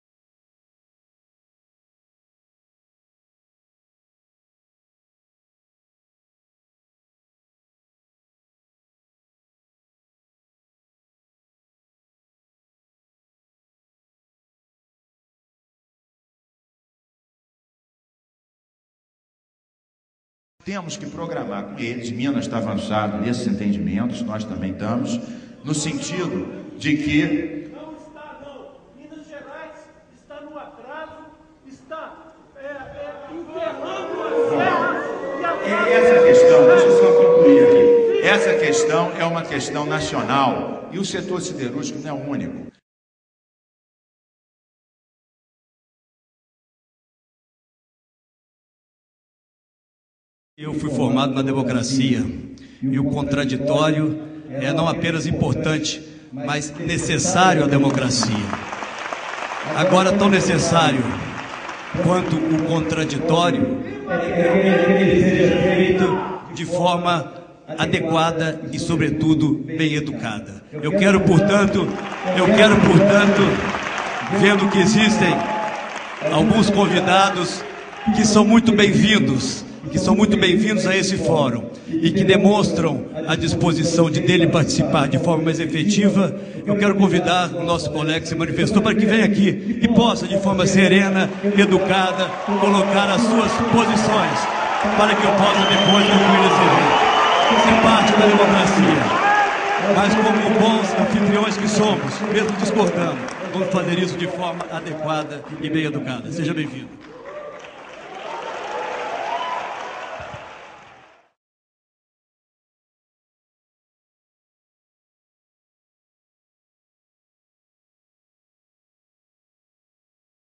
Tenho Muitos Motivos Para Votar no Aécio Neves Mas Basta Este Aqui
Aécio Neves no exercício do mandato de Governador da uma lição de democracia